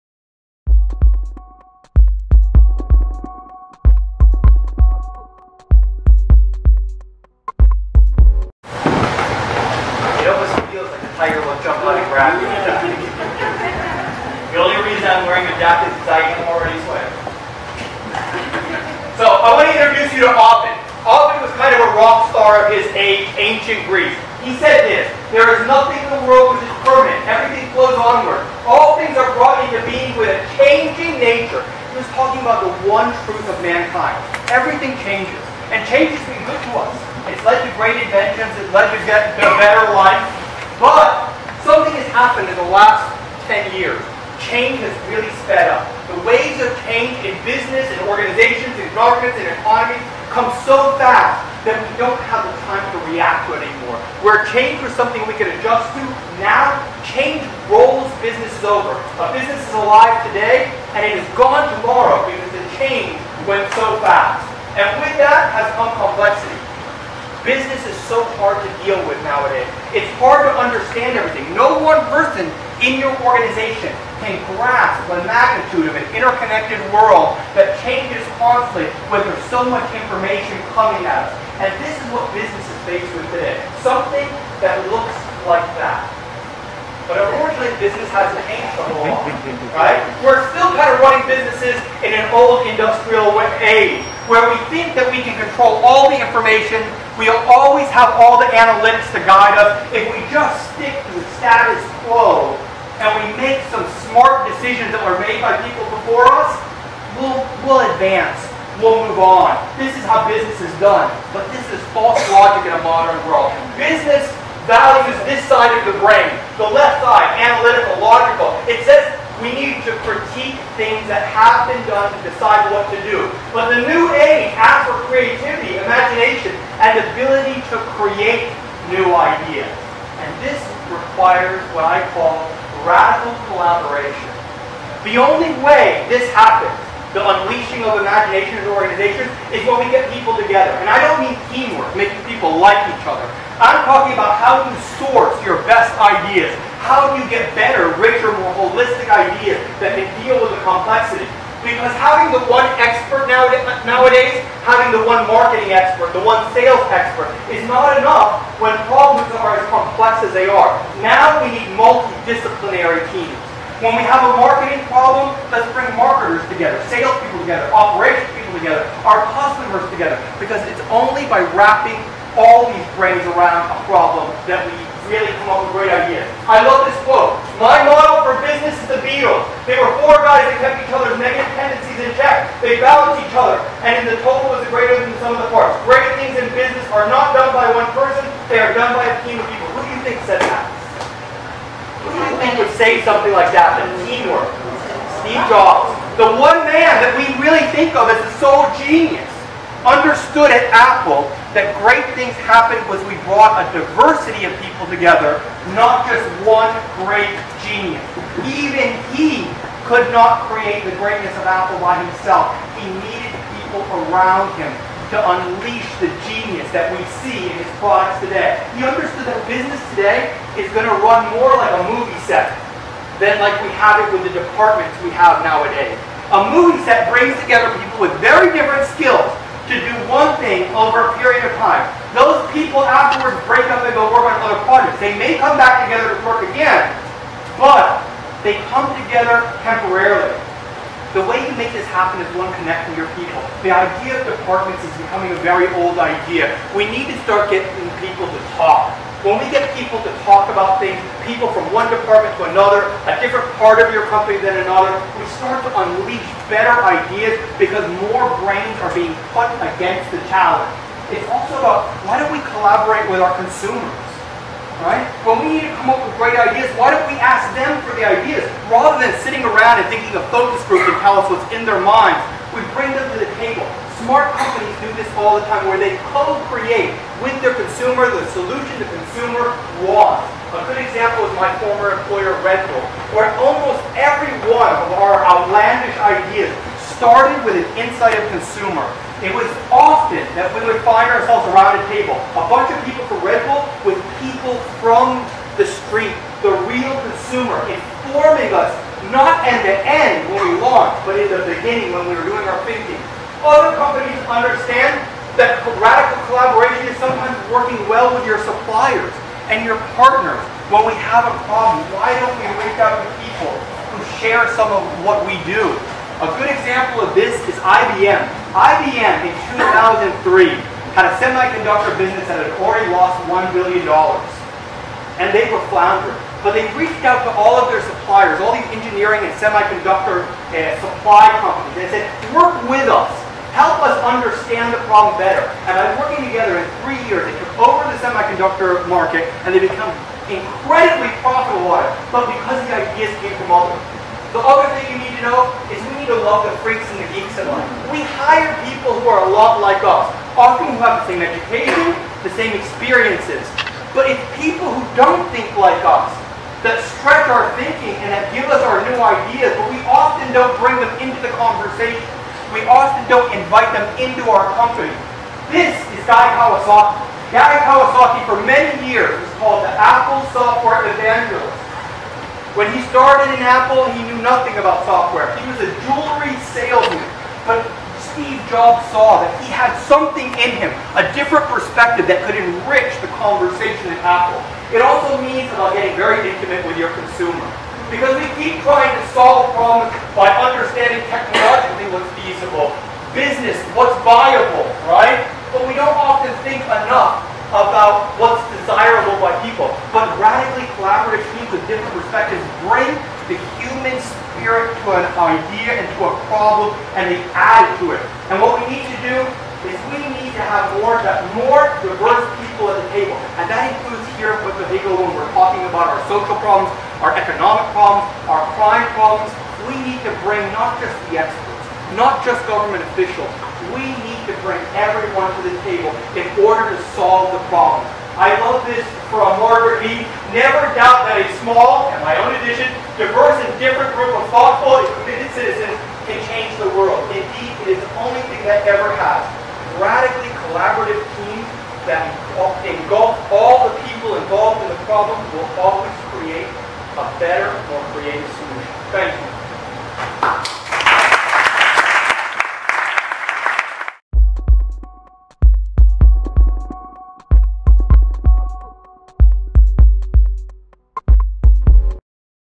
Radically Collaborative by Dana Montenegro
Pecha Kucha #9 @ Seriously Creative. SC's own Dana Montenegro presenting "Radically Collaborative" on March 20, 2012 in San Juan Puerto Rico.